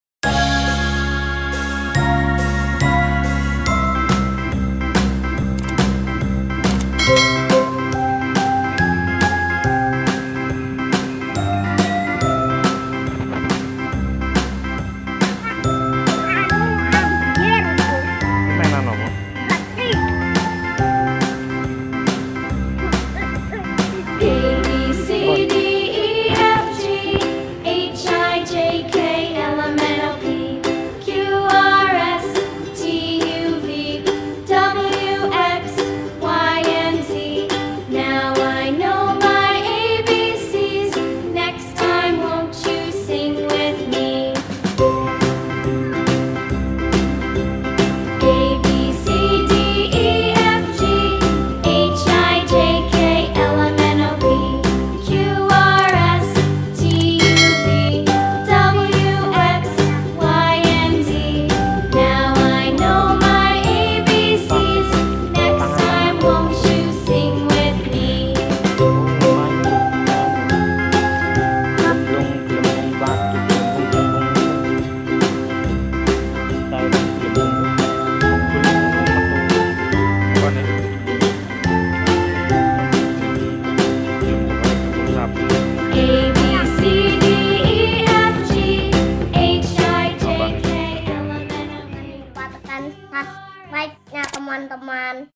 Ke Superindo lihat Mainan
Mas Alif tiap ke supermarket yg dituju selalu rak mainan, lihat satu-satu tanya harganya, kembalikan lagi.
Belinya kapan?
Kalau tabungan(celengan) dirumah penuh baru bisa beli mainan, entah isinya brp, bisa lbh tinggi dr hrgnya tetep hrus beli krn tujuannya untuk melatih menabung dan tiap menginginkan sesuatu tidak instan. Harus ada usaha dulu yaitu nabung.